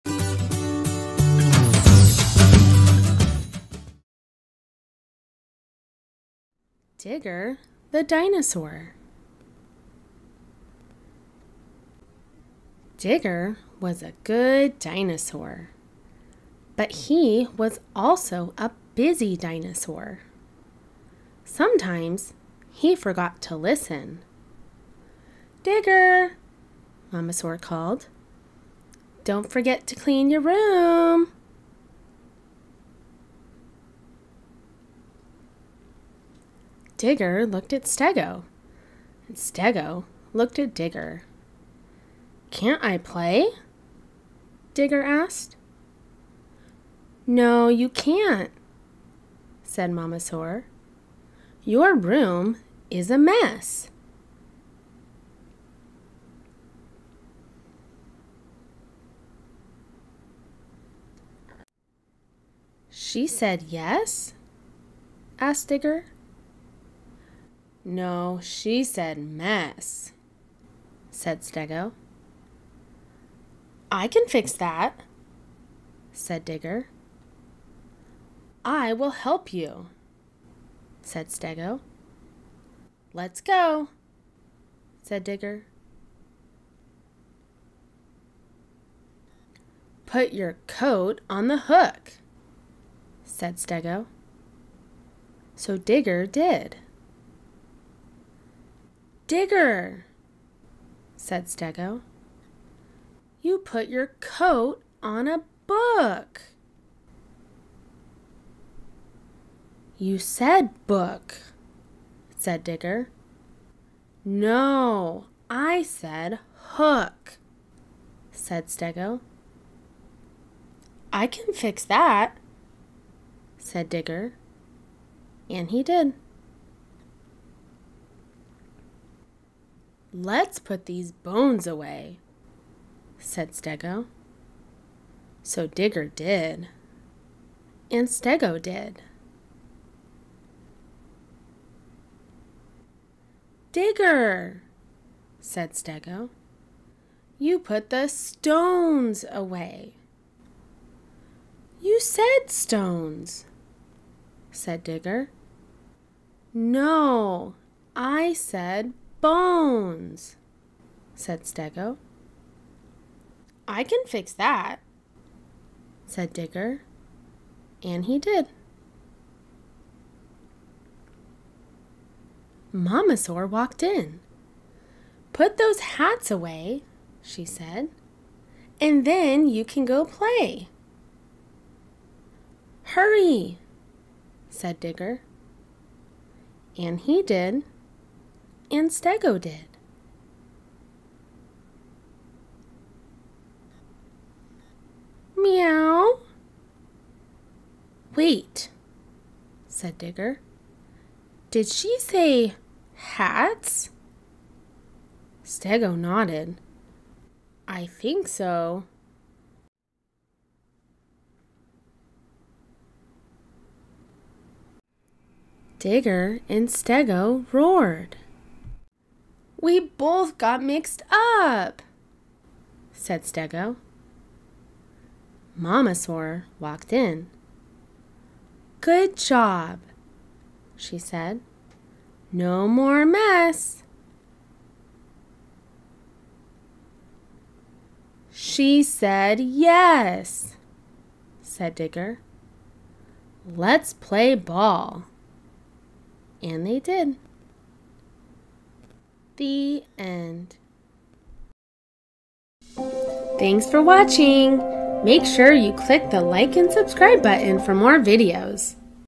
Dinosaur Bedtime Story - Relaxing Book Before Bed | Kids Book
Digger the dinosaur is a busy dinosaur. All he wants to do is play baseball with his friend Stego the stegosaurus, but he has to listen to his mom and clean his room first. Unfortunately, Digger has a hard time picking up his toys because he keeps getting mixed up. My first I can read book is a perfect book to share and read along with a child.